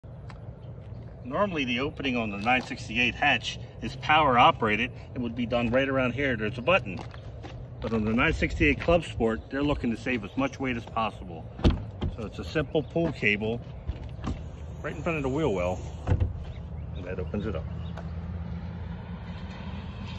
Did you know? How to open Porsche 968 Clubsport's hatch
In the name of saving weight, Porsche used a different mechanism to open the 968 Clubsport's hatch. Here's how it works.